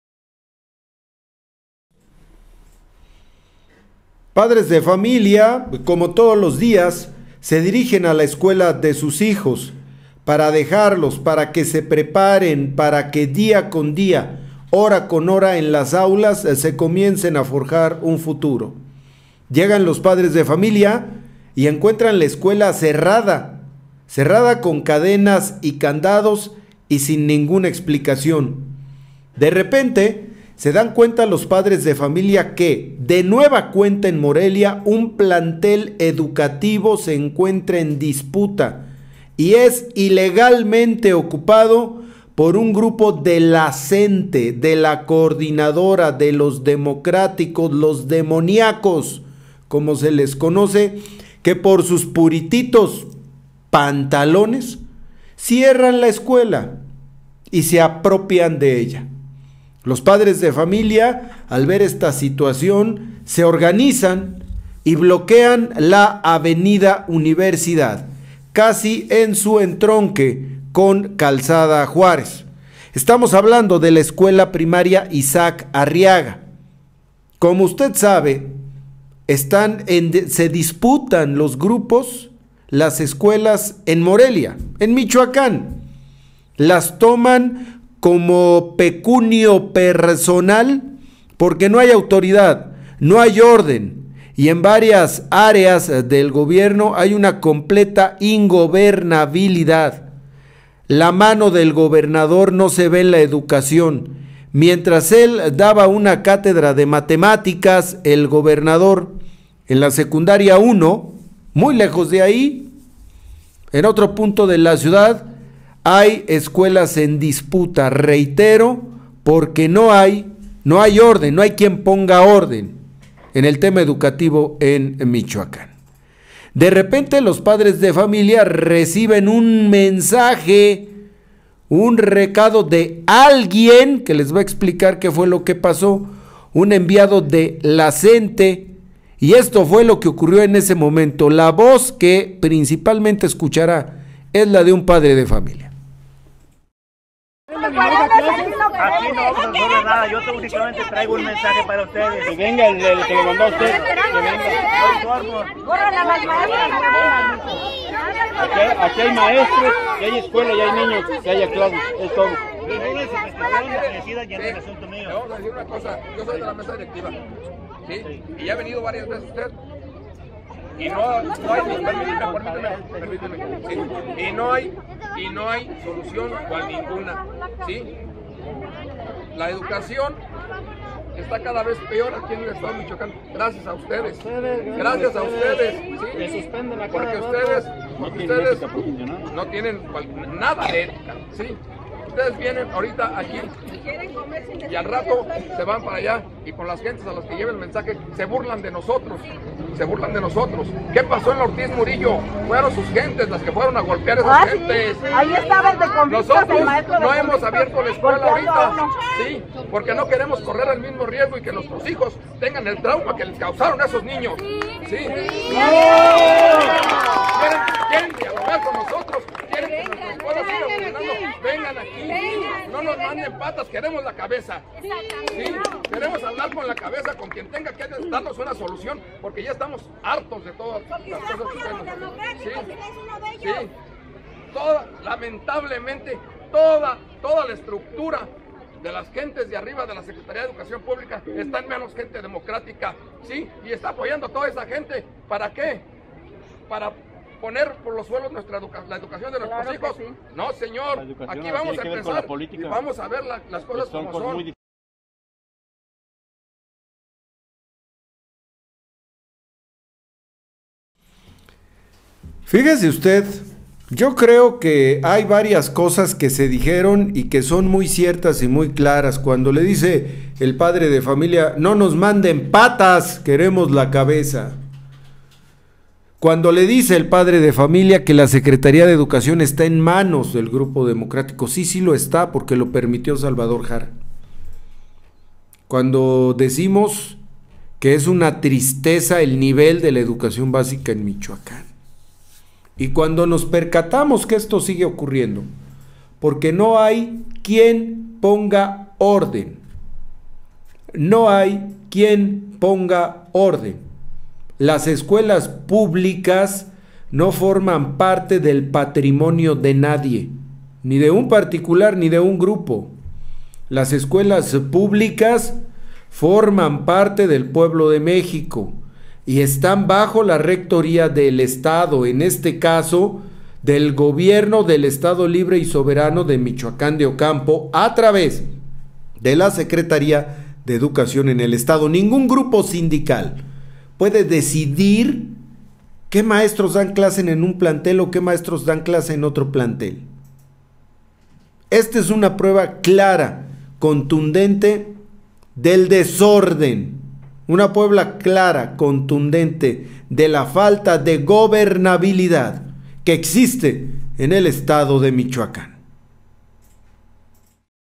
NUEVO ENFRENTAMIENTO ENTRE "PROFES" DE LA CNTE Y P
DE NUEVA CUENTA SIN ARGUMENTOS LA CNTE CIERRA OTRA ESCUELA PRIMARIA EN LA CIUDAD DE MORELIA, AL CUAL DISGUSTA A LOS PADRES DE FAMILIA POR QUE SUS HIJOS PIERDEN CLASES - Captured Live on Ustream